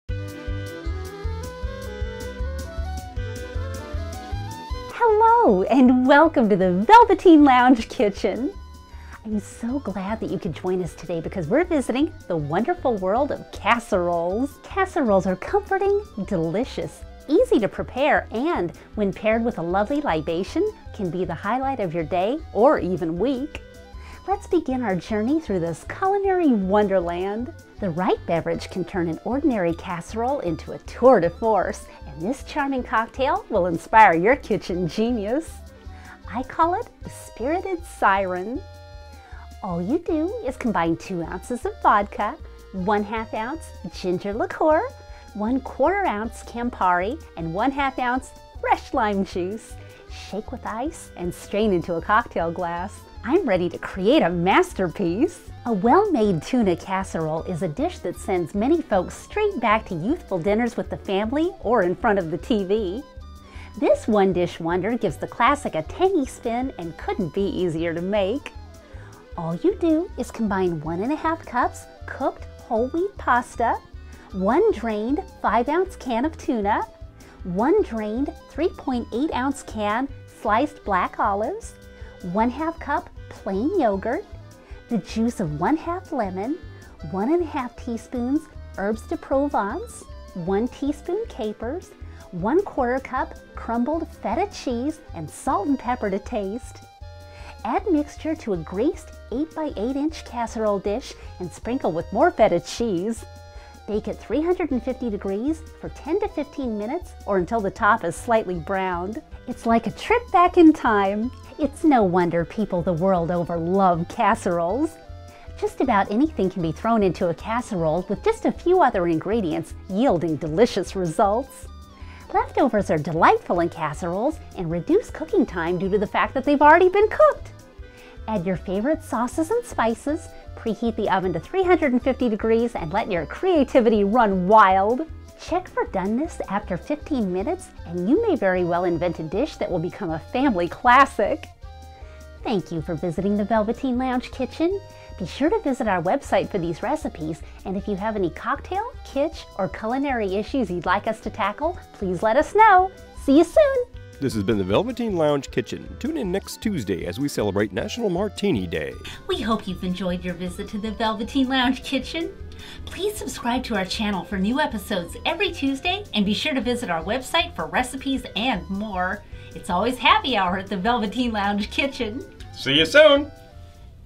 Wonderful World Of Casseroles! Velveteen Lounge Kitsch-en
Recipes and tips for your journey through the culinary wonderland of casseroles!
Recipes and more on our site! velveteenloungekitsch-en.com
Please let us know if you have any cocktail, cooking or kitsch-related issues you'd like us to tackle!